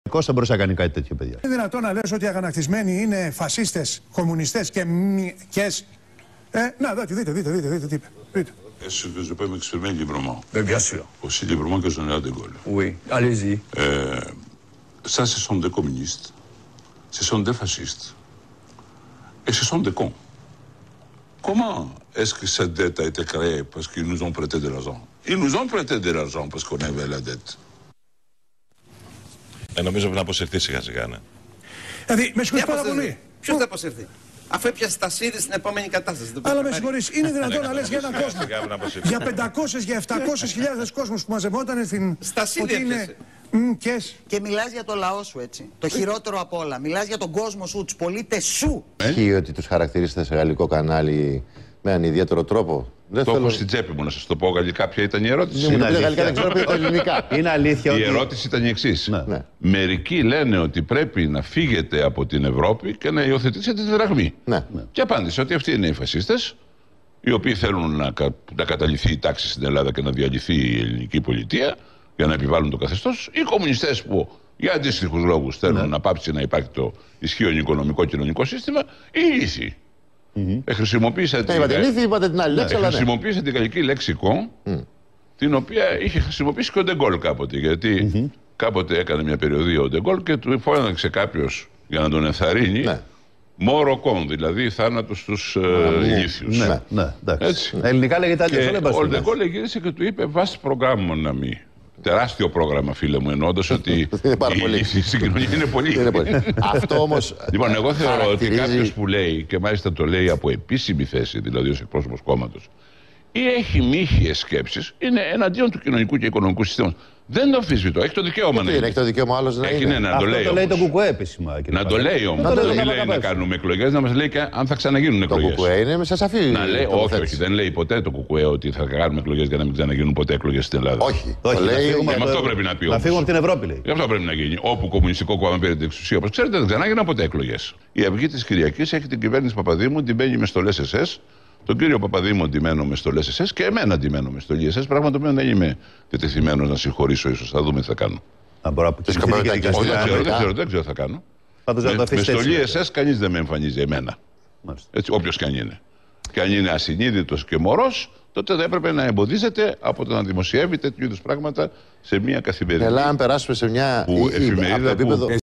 Συνεχίζει το παραλήρημα ο Πάγκαλος
Ο Πάγκαλος μετά τις προκλητικότατες δηλώσεις σε γαλλικό κανάλι,συνέχισε το παραλήρημα του και εντός Ελλάδος στην τηλεόραση του ΣΚΑΪ